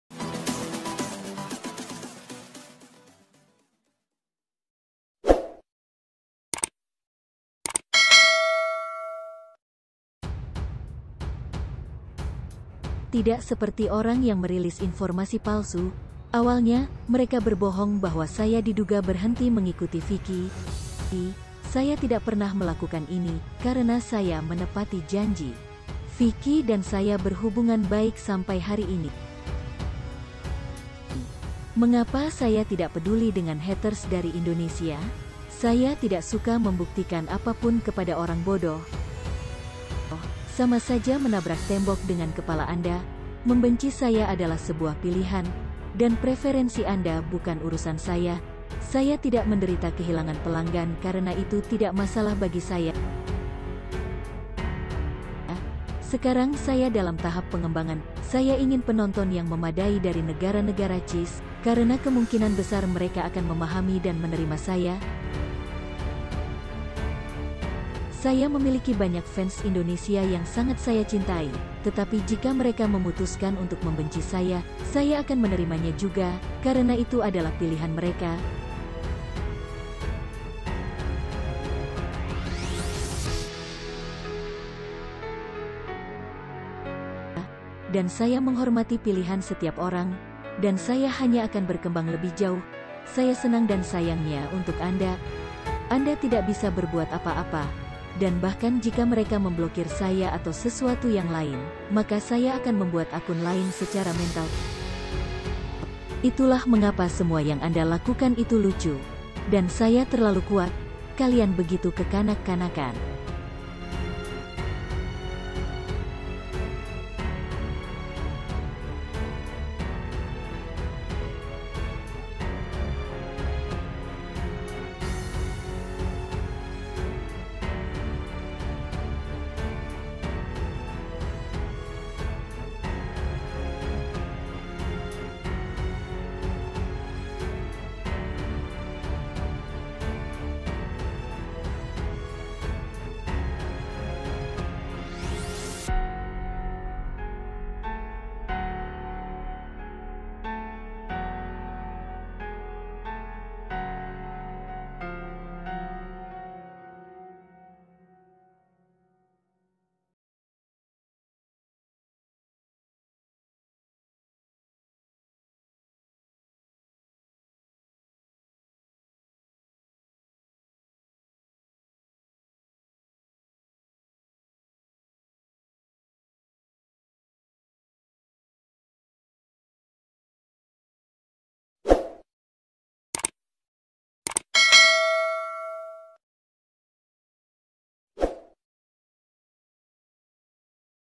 DAYANA GER4M! Postingan dayana makin p3rkeruh suasana
Akhirnya dayana kembali memposting isi hatinya di instagram.
Simak terjemagannya👆
#+62